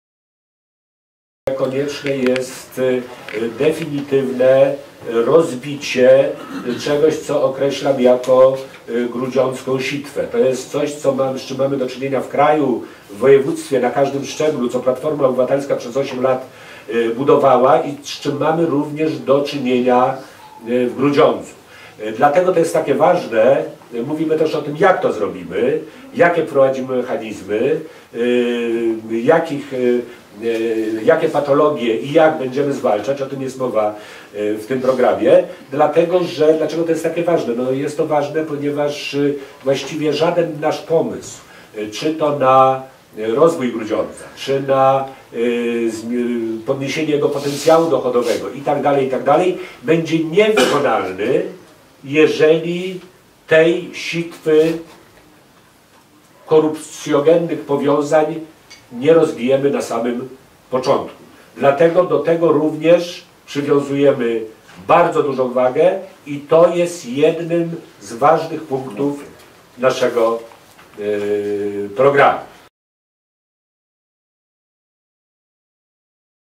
“Konieczne jest rozbicie grudziądzkiej sitwy”
“Konieczne jest rozbicie czegoś, co określam grudziądzka sitwą, czegoś z czym mamy do czynienia w kraju (..) co Platforma przez osiem lat budowała i z czym mamy do czynienia w Grudziądzu” - mówi Andrzej Wiśniewski